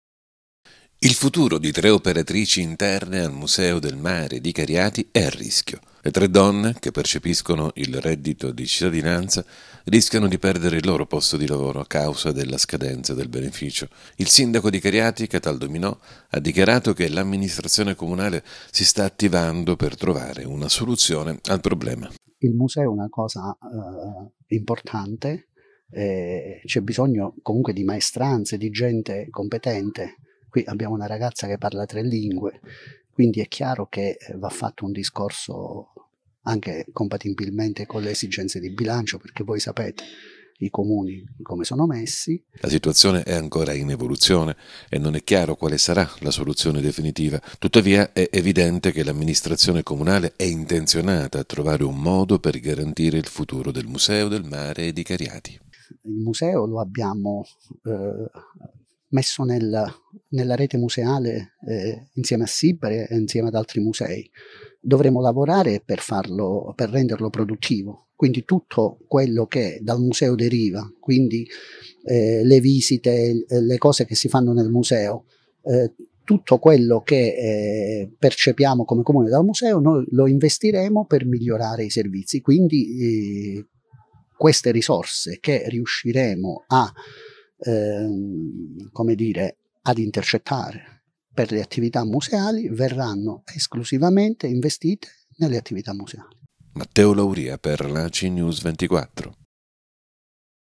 Museo del Mare di Cariati, a rischio il posto di lavoro di tre operatrici
La situazione futura di tre operatrici interne al Museo del Mare di Cariati è incerta e a rischio. Le tre beneficiarie del reddito di cittadinanza, si trovano di fronte alla minaccia di perdere il loro impiego a causa della scadenza di tale supporto finanziario